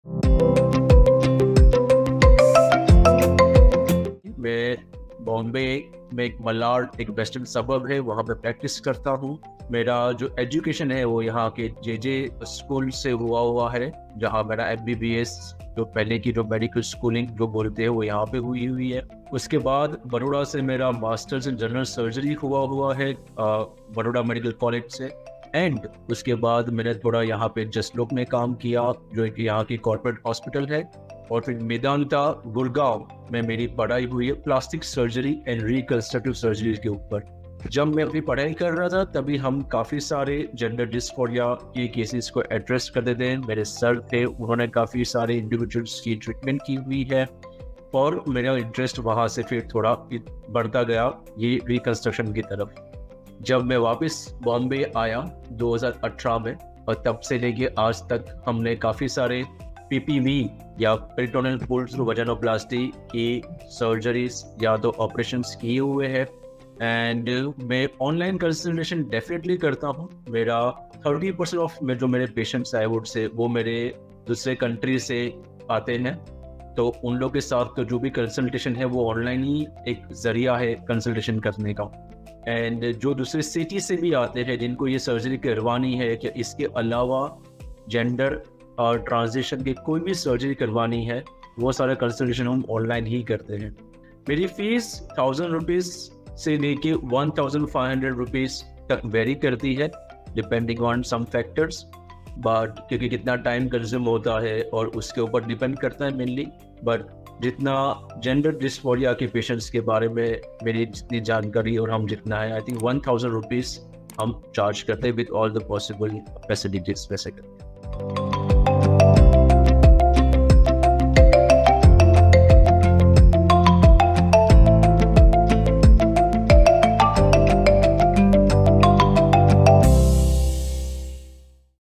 Who is Dr Umang Kothari? How can I get a consultation with him?
Who is Dr Umang Kothari? In this video, we will talk to Dr Umang Kothari who is an experienced surgeon and he will tell us more about his experience and work. If you like him, we also have a segment where you can find out how you can get a consultation with him.